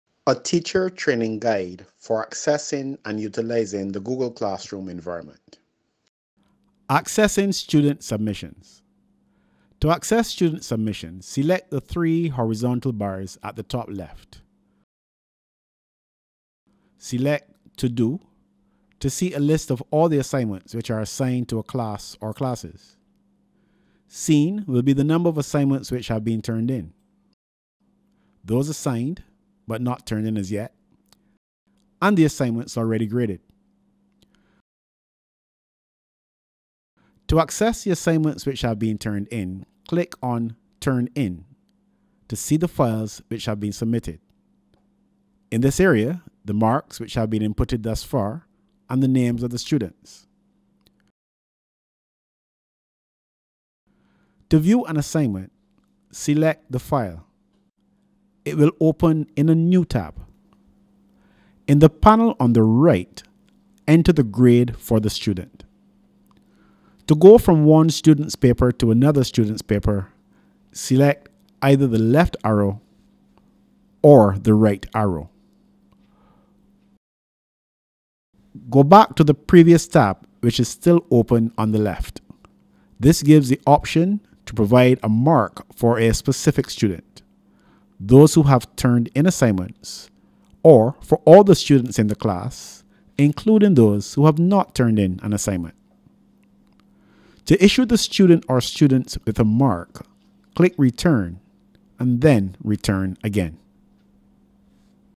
8 of 8 Accessing student submissions
A teacher training guide for accessing and utilizing the google classroom environment